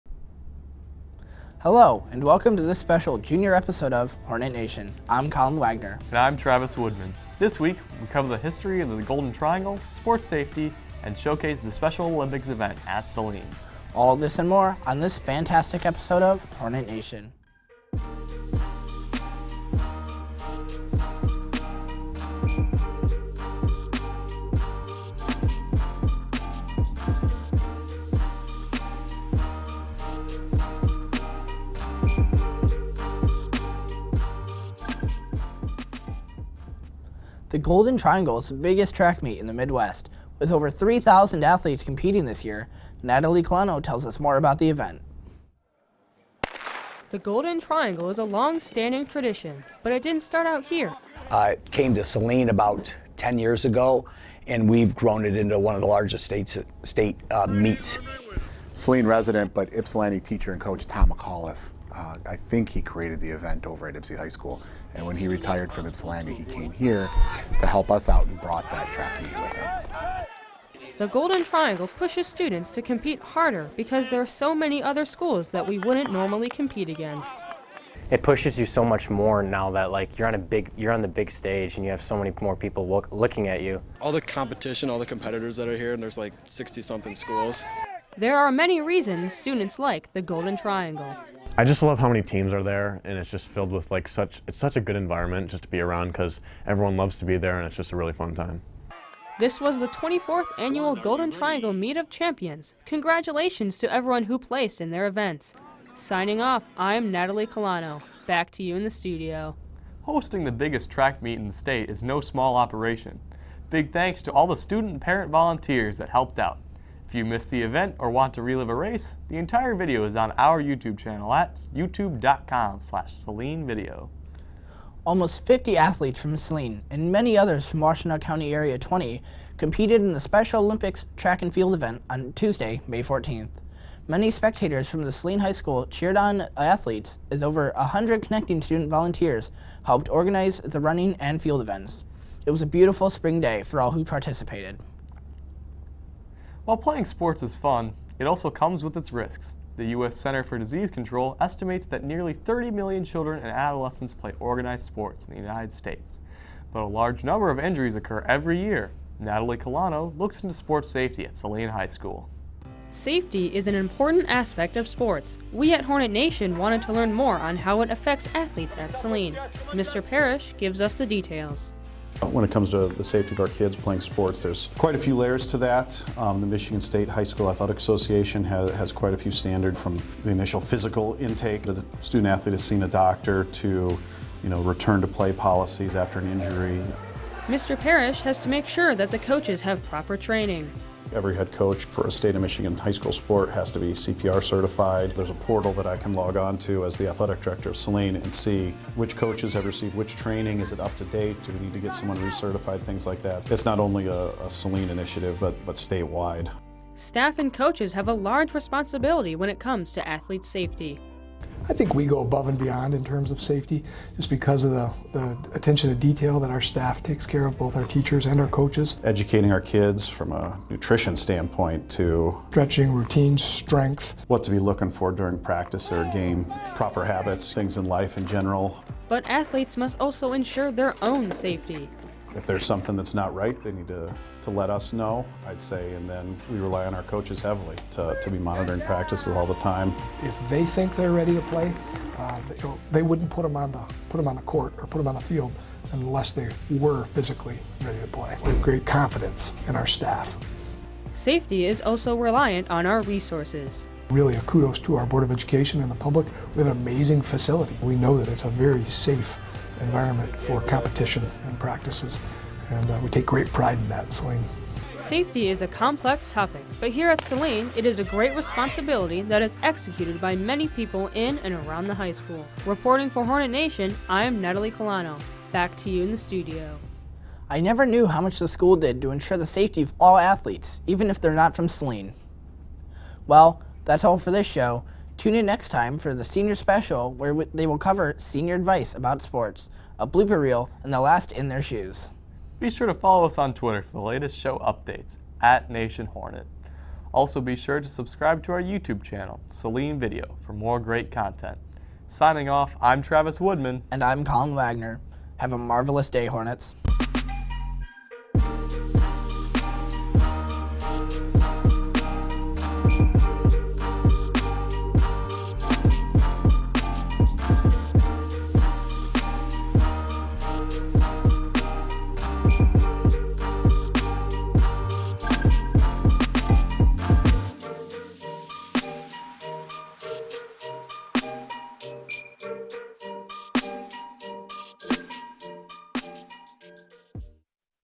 HN 5.15
Hornet Nation - May 15, 2019

Hosts:
Collin Wagner
Travis Woodman

Features:
Golden Triangle Track Meet
Sports Safety
Special Olympics Track and Field Event